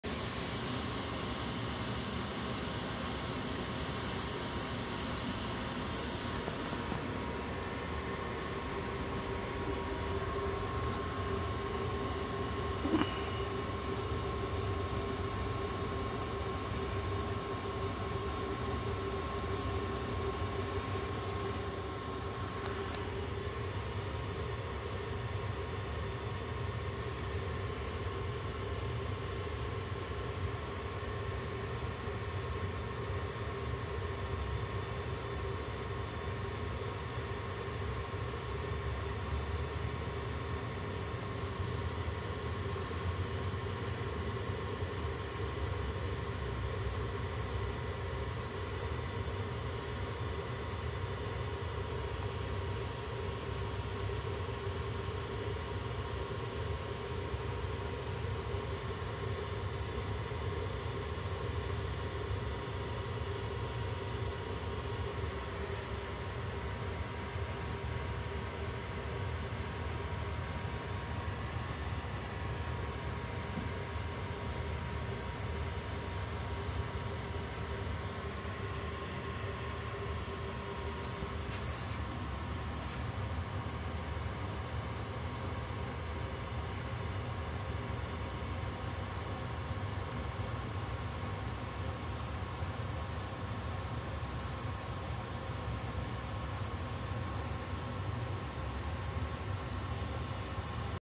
Daddy long legs vs wasp
War between Daddy long leg spider and a wasp caught in the spider's web.